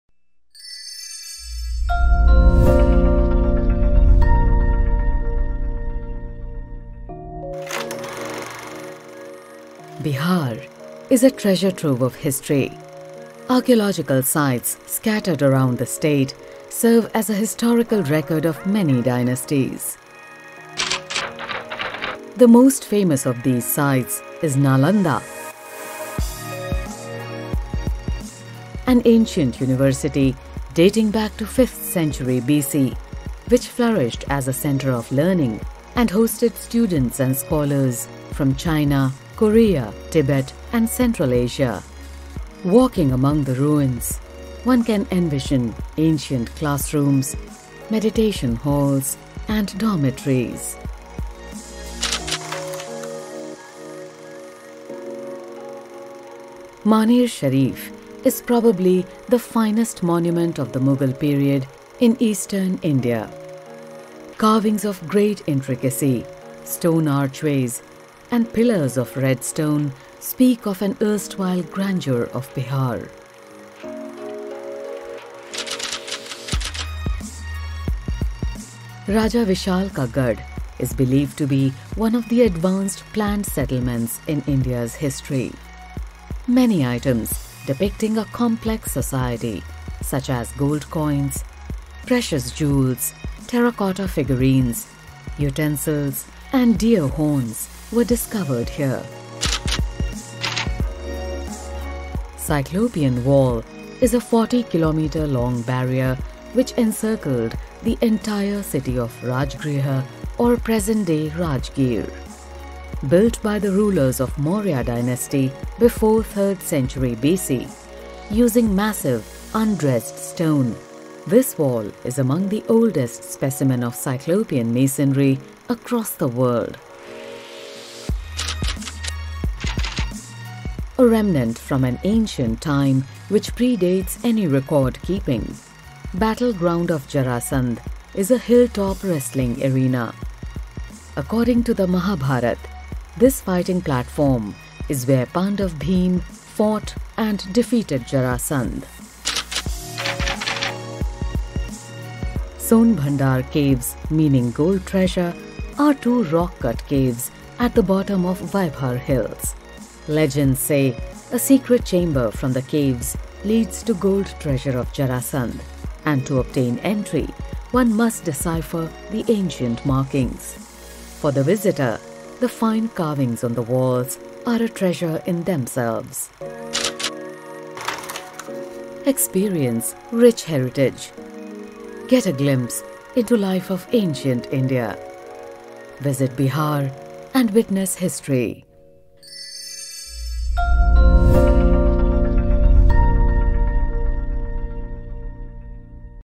World Heritage Week 2024 | विश्व धरोहर सप्ताह | UNESCO | Discover and Experience Diversity
An archaeological site is a vault containing rich historical and cultural relics. It is significant not only for historical research but also for communal and economic benefits. Bihar is home to several archaeological monuments and is one of the world's oldest inhabited places. Sages, emperors, and commoners from all times have passed through this lovely place
and erected culturally diverse edifices that are now tourist attractions.
Today, we'll show you some of Bihar's most well-known archaeological sites. These Bihar archaeology sites are not to be missed -
● Nalanda Ruins, Nalanda
● Jarasandh Ka Akhara, Rajgir
● Maner Sharif, Patna
● Cyclopean Wall, Rajgir
● Raja Vishal Ka Garh, Vaishali
● Son Bhandar Caves, Rajgir